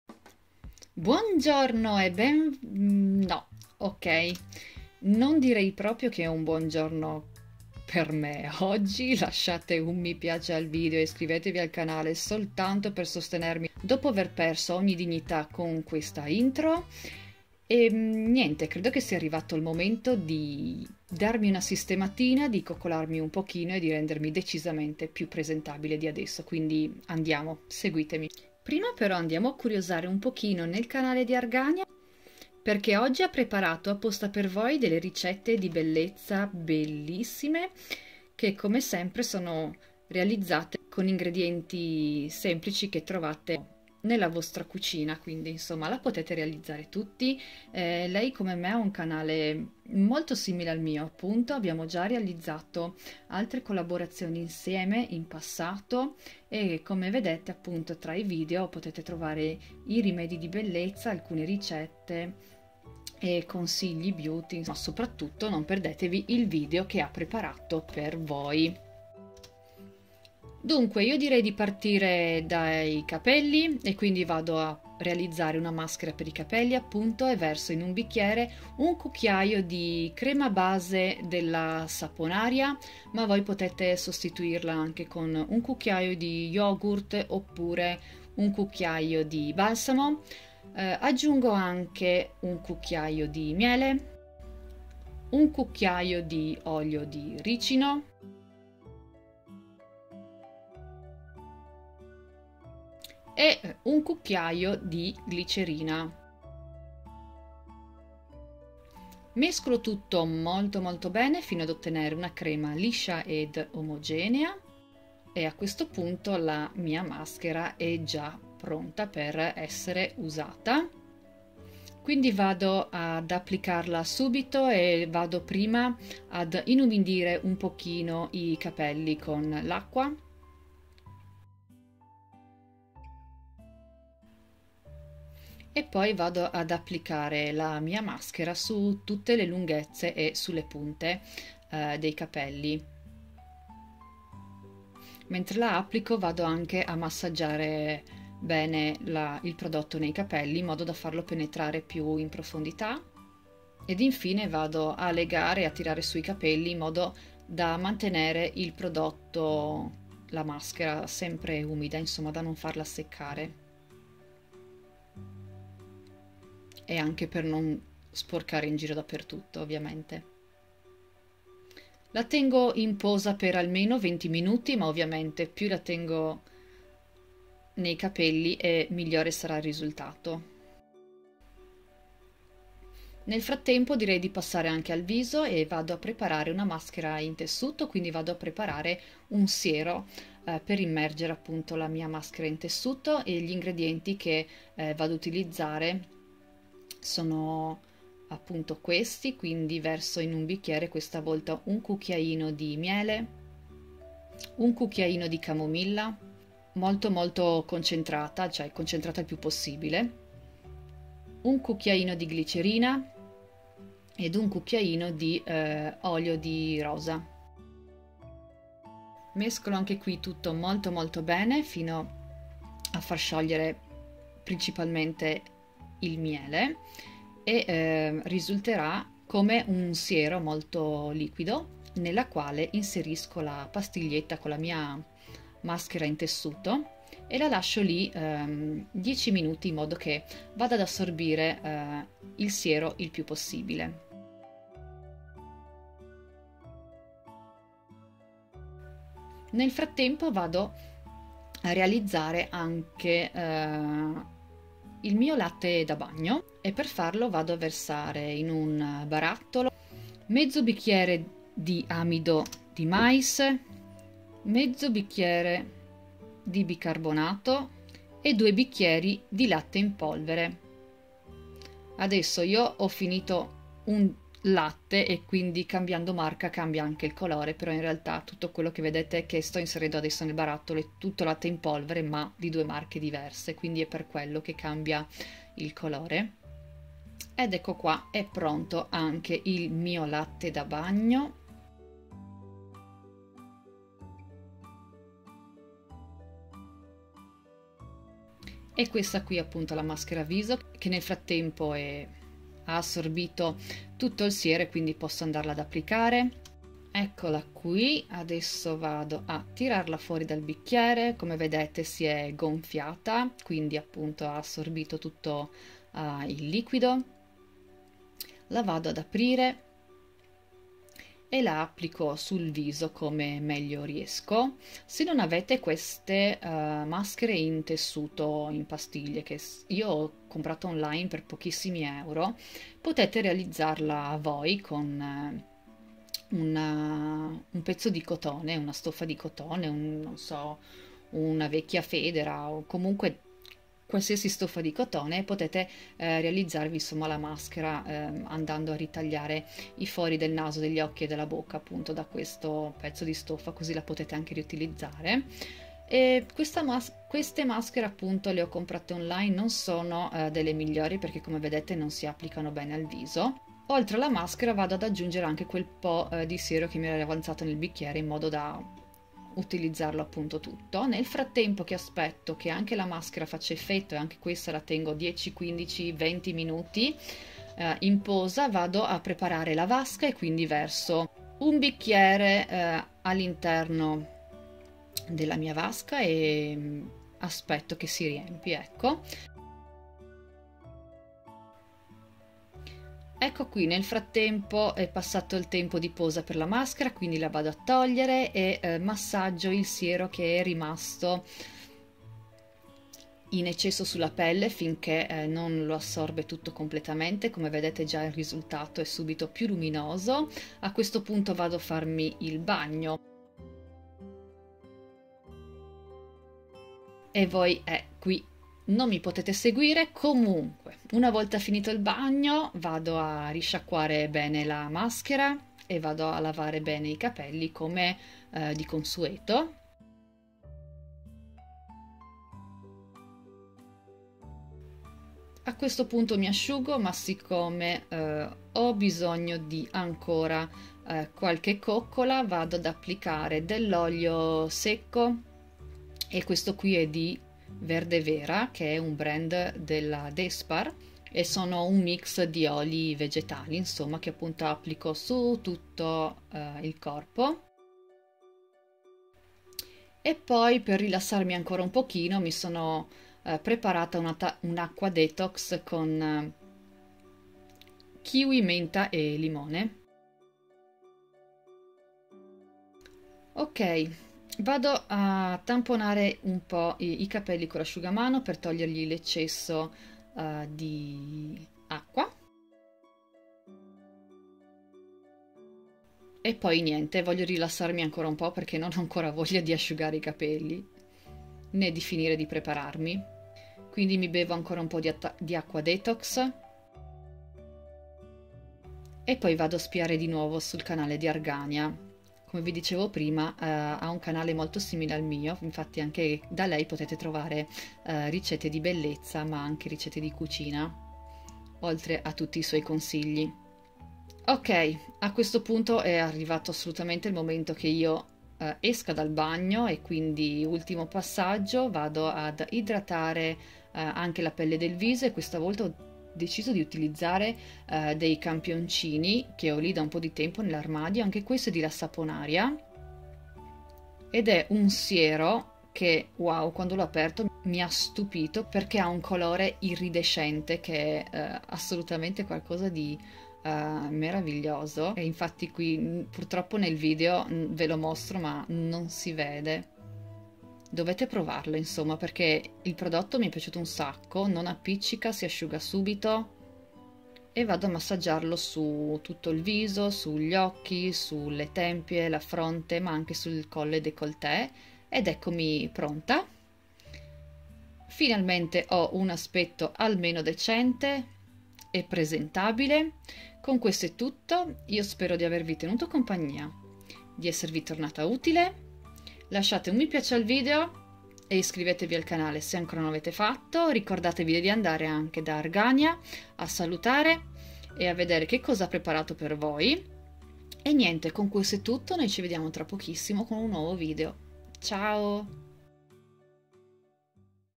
3 RICETTE DI BELLEZZA FAI DA TE PER UN BAGNO RILASSANTE - latte, miele, camomilla
IN QUESTO VIDEO 3 RICETTE DI BELLEZZA DA REALIZZARE FACILMENTE CON INGREDIENTI CHE AVETE GIÀ IN CASA:
* BAGNO AL LATTE
*MASCHERA VISO IN TESSUTO CAMOMILLA E MIELE
*MASCHERA CAPELLI AL MIELE

FATEMI SAPERE NEI COMMENTI QUALE PREFERITE 😘